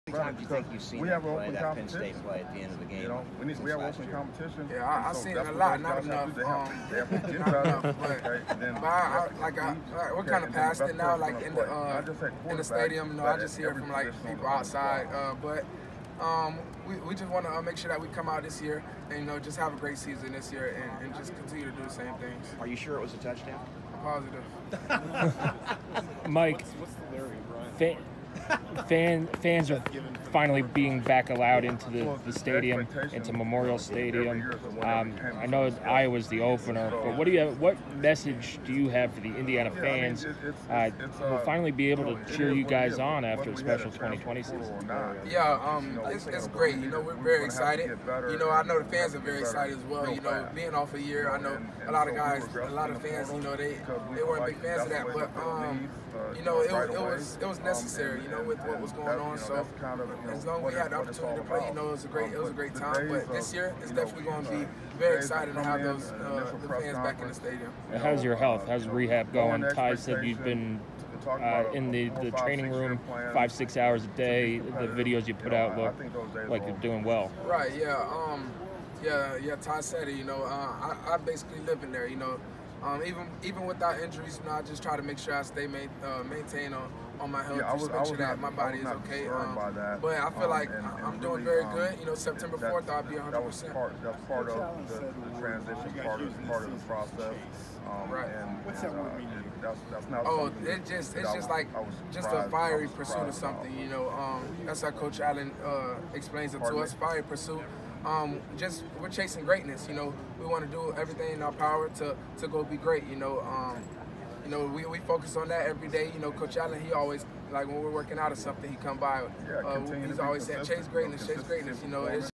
2021 Big Ten Media Days— IU QB Michael Penix Jr.
Indiana quarterback Michael Penix Jr. addresses the media at Big Ten Media Days